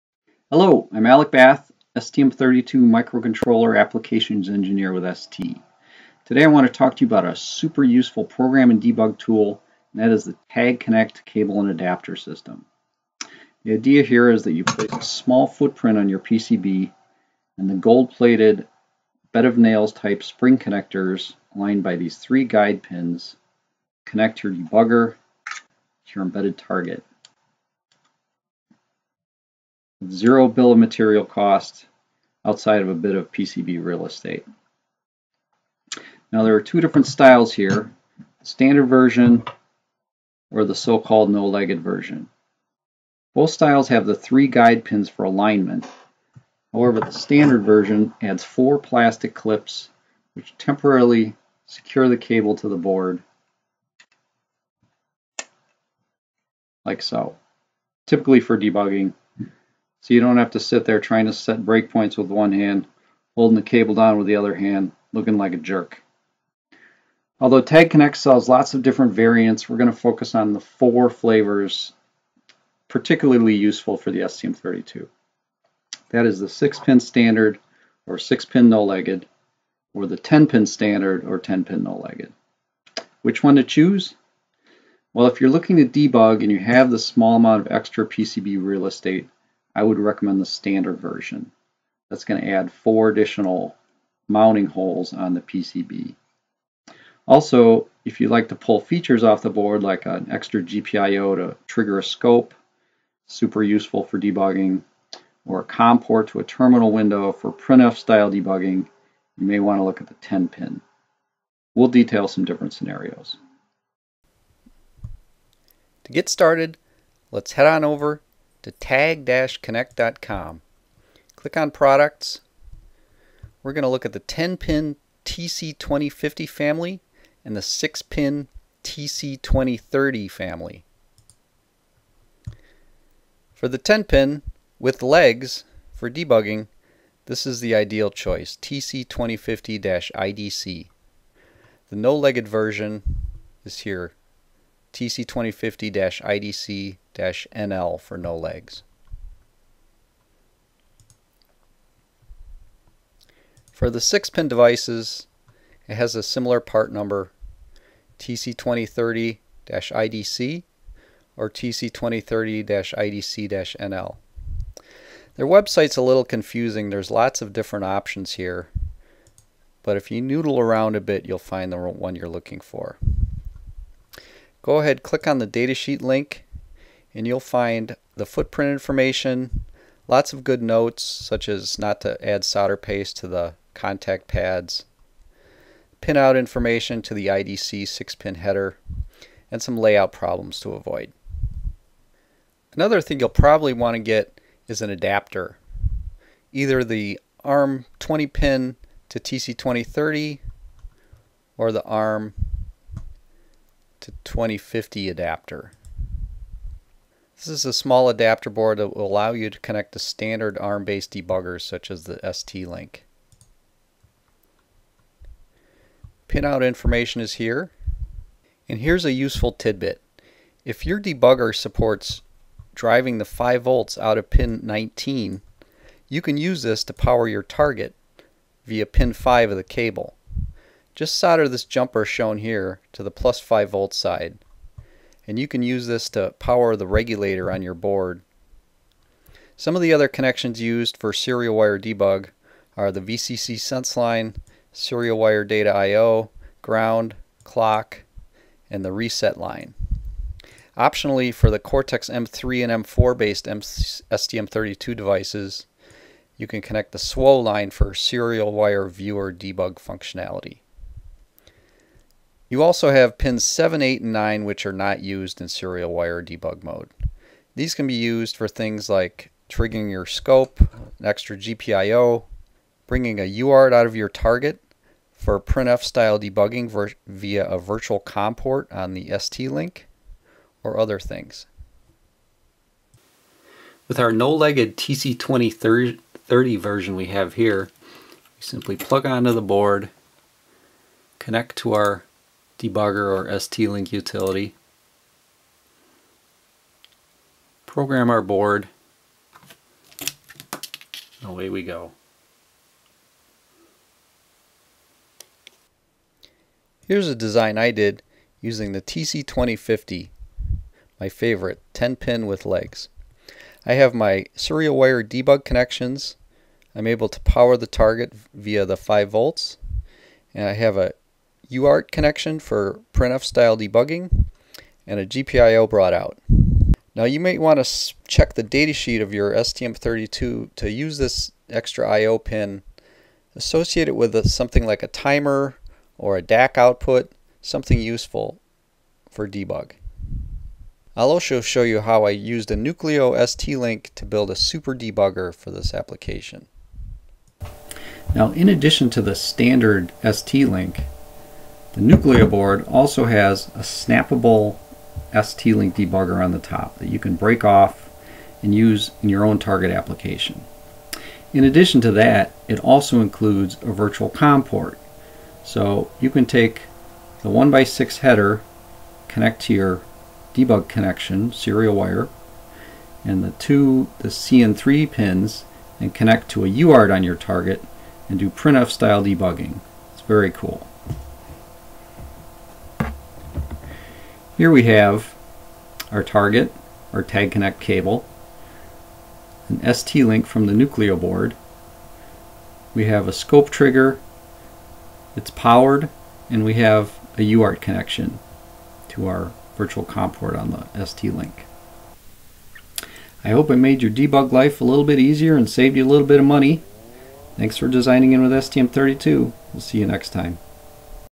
STM32 debugging with the Tag-Connect system
Improve your STM32 debug environment and save some BOM cost with the Tag-Connect system!